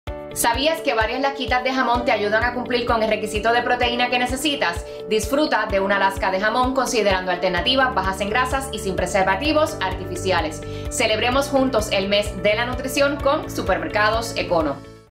Mes de la Nutrición: El Jamón
Seguimos celebrando en Mes de la Nutrición con la Lcda. Getsení Rodríguez y Oscar Mayer.
¿Sabías que varias lascas de jamón te ayudan a cumplir con el requisito de proteína que necesitas? Considera alternativas bajas en grasas y sin preservativos artificiales.